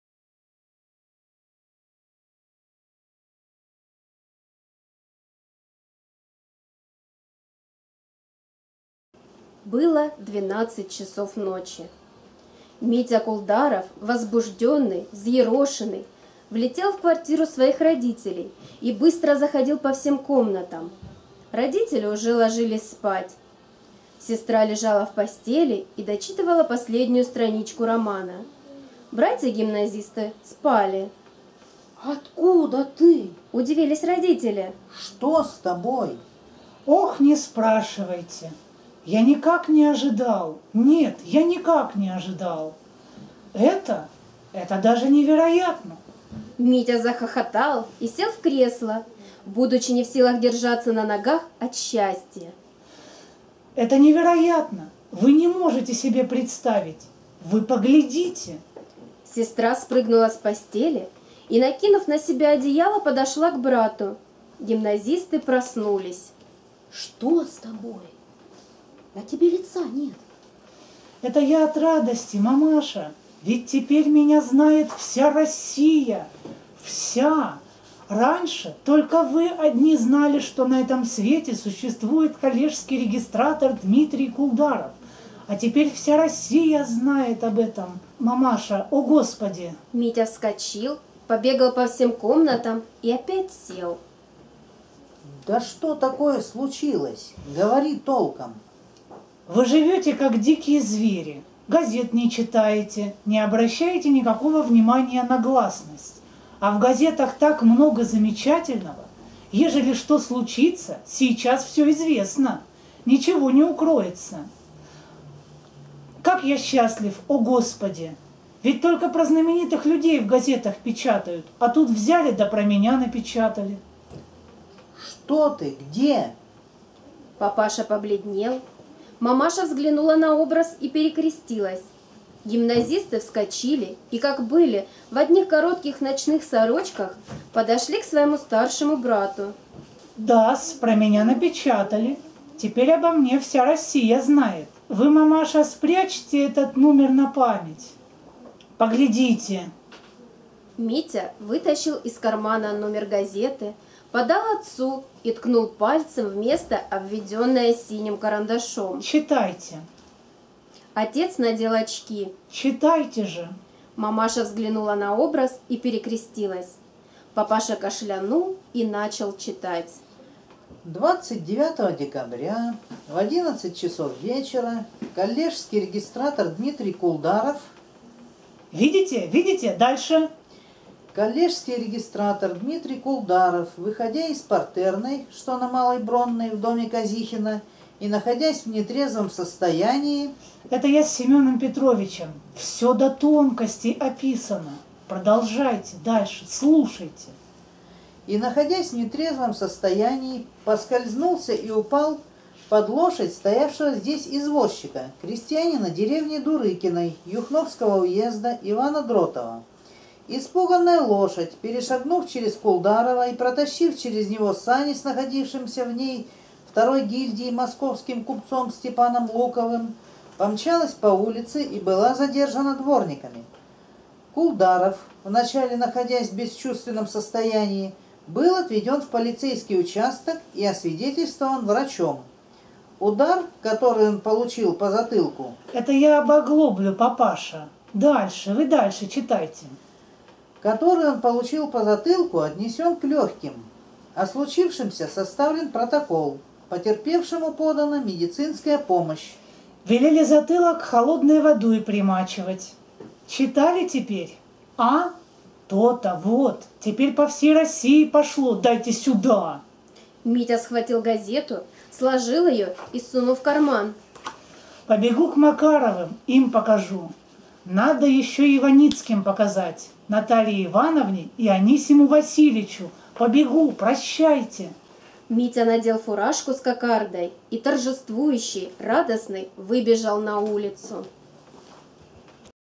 Рассказ А. П. Чехова "Радость"
Незрячие читатели Ставропольской краевой библиотеки для слепых и слабовидящих имени В. Маяковского читают по ролям рассказ Антона Павловича Чехова "Радость".
Рассказ читают: Митя Кулдаров  - Александр Лещёв; Мамаша - Елена Гузева; Папаша - Василий Павлов; Голос автора - Анастасия Пустовалова.